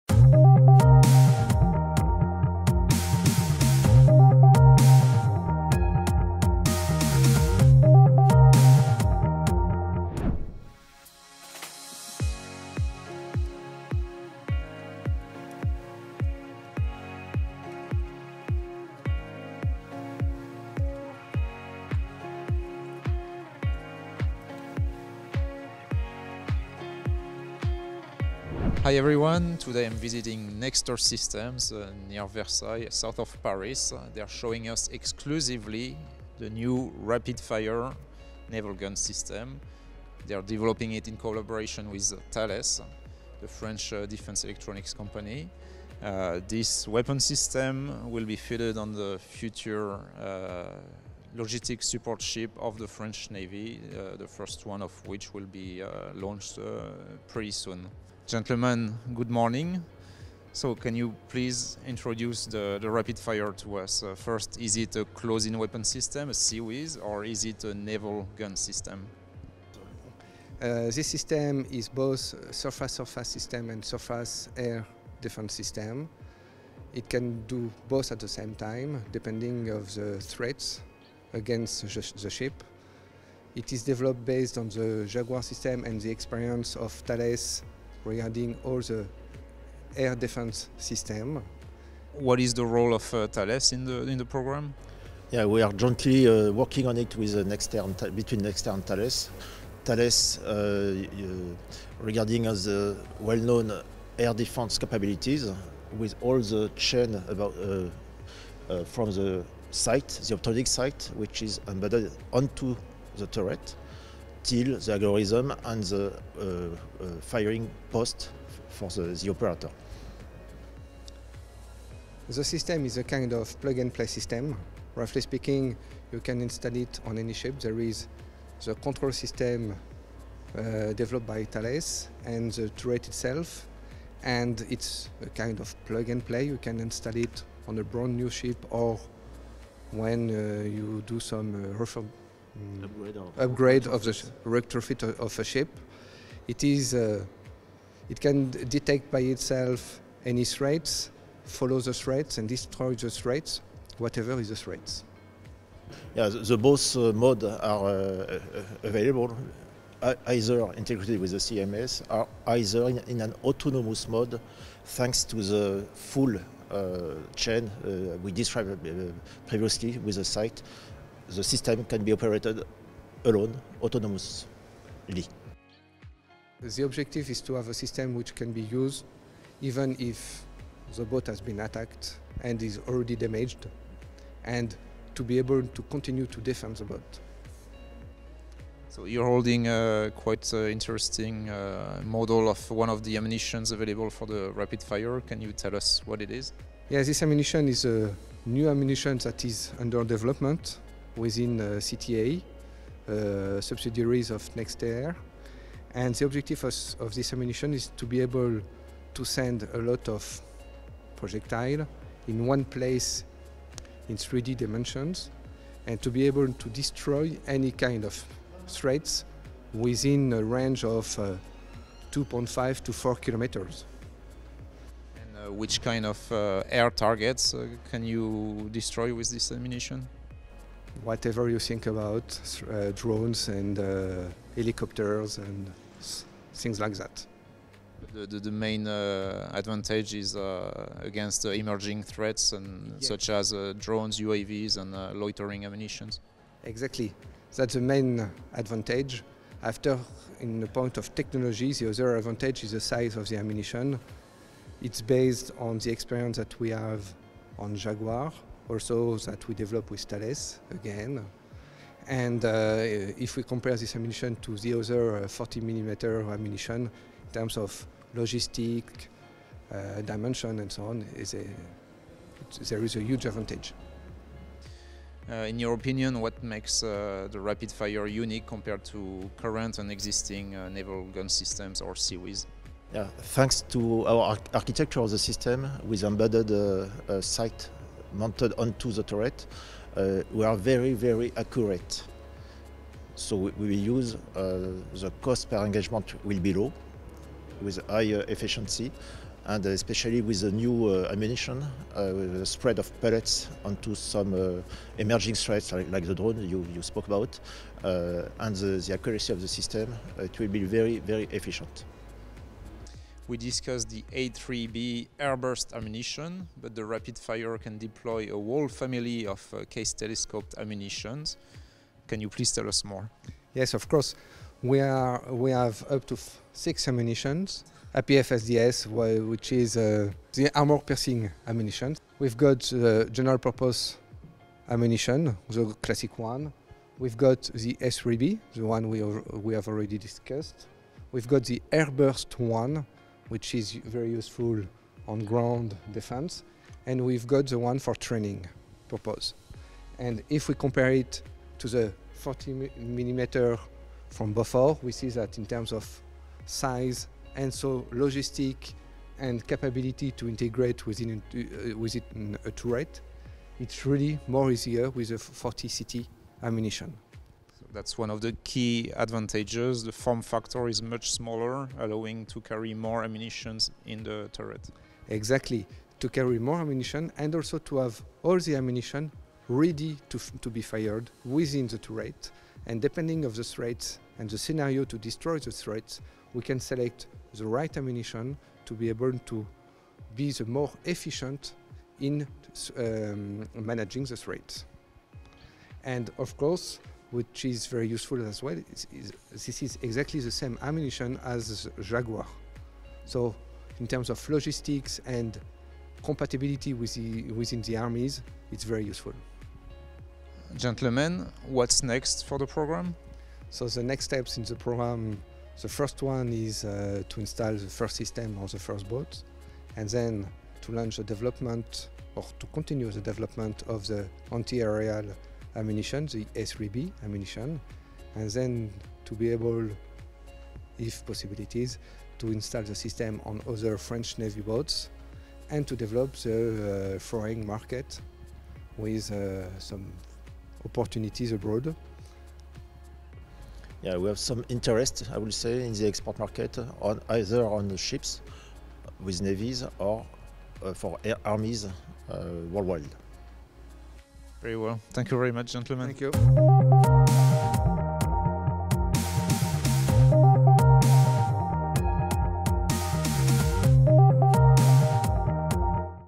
RAPIDFire: The New French 40mm Naval Gun System by Nexter & Thales
Nexter and Thales presented exclusively for Naval News their new RAPIDFire remotely operated naval gun system. This complete solution includes the 40 mm CTA gun, an integrated fire control system, an operator console and a sighting system.

The RAPIDFire system developed by the consortium formed by Thales and Nexter has been chosen by the French defence procurement agency (DGA) to equip future French Navy vessels (the Chevallier-class replenishment tankers also known as BRF and the future class of OPV) with new artillery. This new system will provide the Navy’s surface vessels with an effective close-in defence capability against modern air and surface threats.

RAPIDFire incorporates the new medium calibre 40 mm gun developed by CTAI, the international subsidiary of Nexter Systems and BAE Systems. The system is compatible with the full range of ammunitions developed for land forces programmes. For naval air defense, a development and proudctiion contract for a future smart ammunition known as Anti Aerial Airburst (A3B) round is expected to be awarded this year.